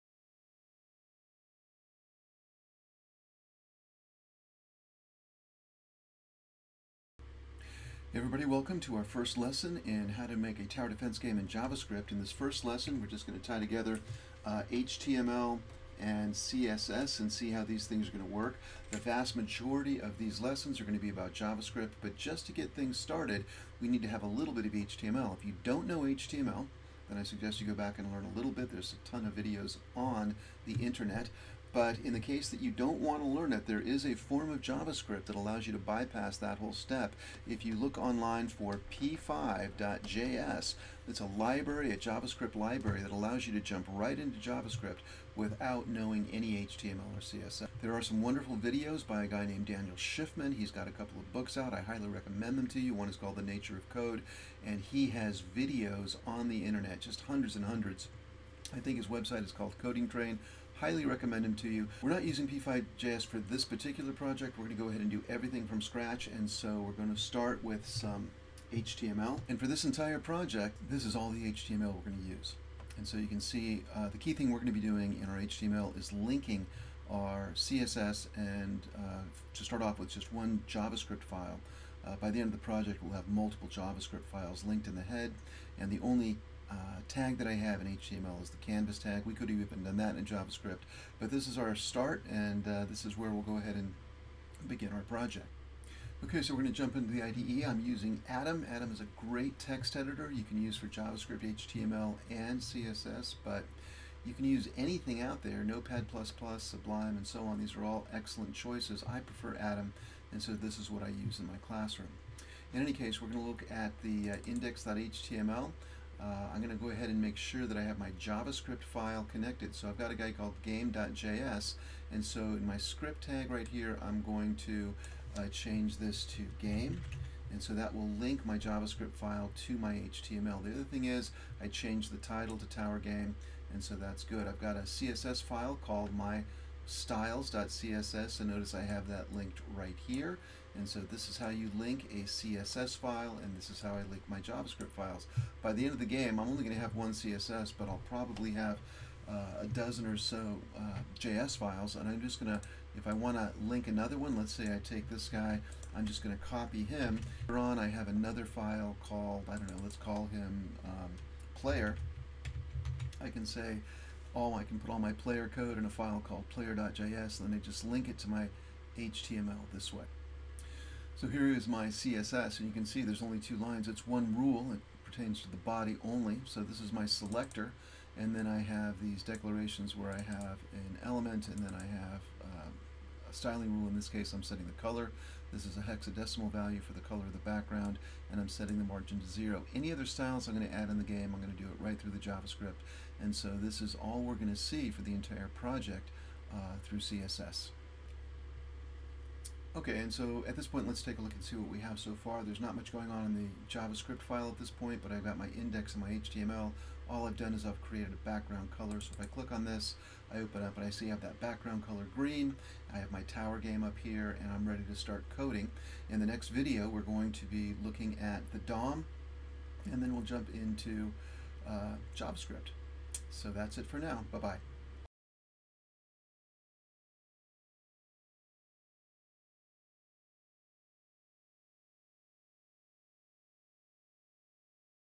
1.1 JavaScript Tower Game ::HTML and CSS setup
This game is designed for post APCS kids who have completed a year of Java and want to try Javascript